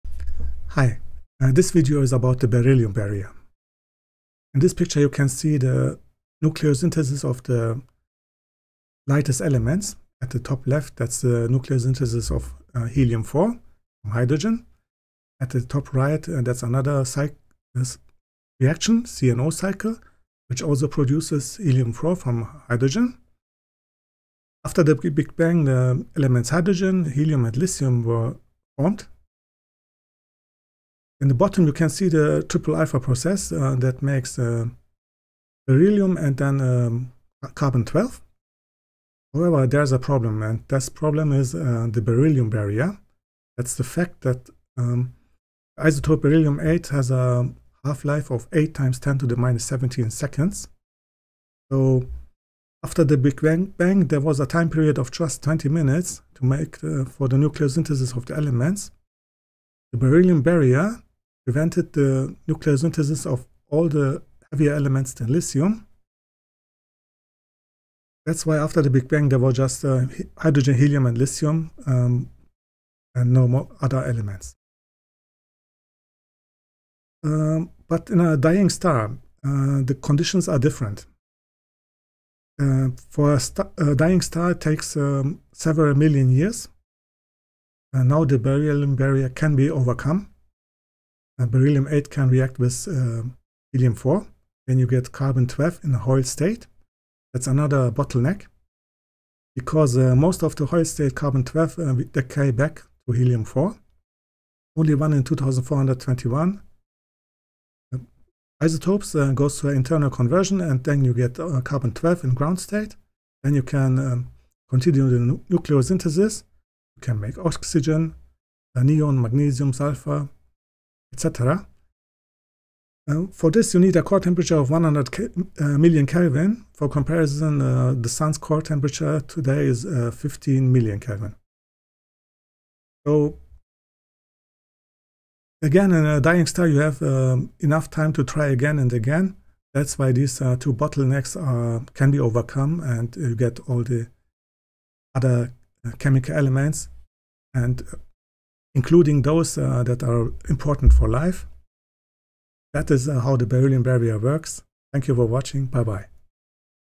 The Beryllium Barrier: What It Is and Why It Matters
The Be barrier prevented the nucleosynthesis of the elements other than H, He and Li right after the Big Bang 13.8 Gyrs ago.